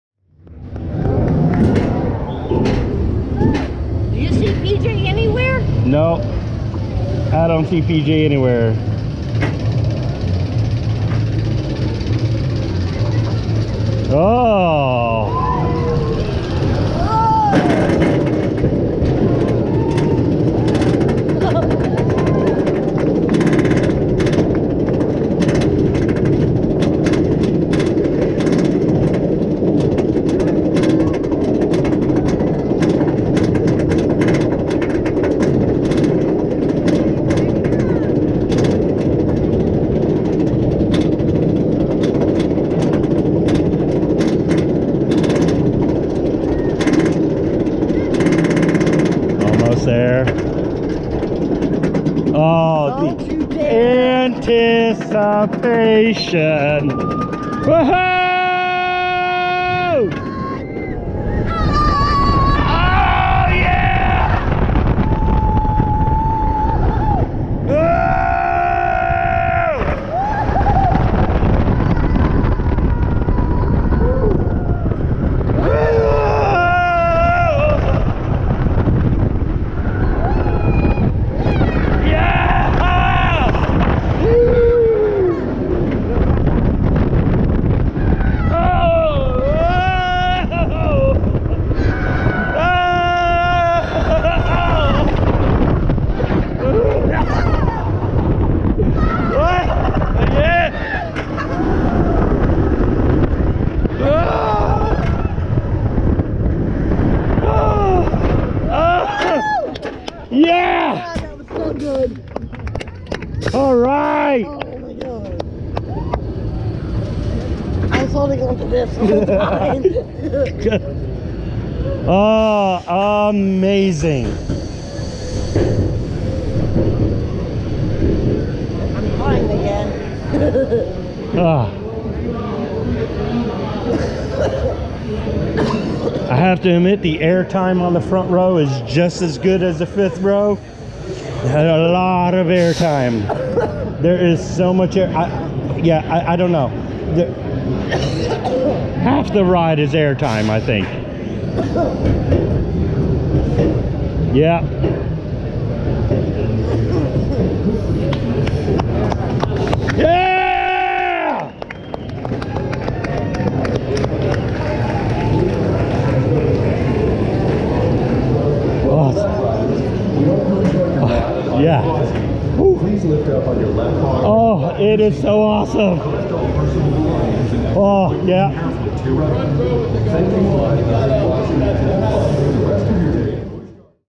Iron Gwazi Front Row POV
Busch Gardens Tampa is opening their newest roller coaster Iron Gwazi!
Ride with us on the tallest, steepest, fastest, hybrid coaster in North America!
206 feet 91 degree drop that will take you to 76 MPH! There is a thrill a second on this RMC!

**videos taken with approved chest/wrist mounted GoPro, always follow safety rules when filming!**